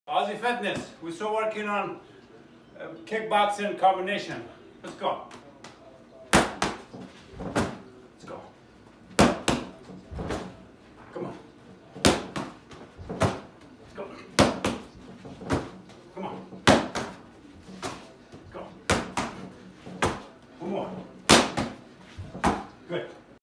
Kickboxing
The Coronavirus is everywhere outside, and this is another way of working out and is also another way of motivation at home.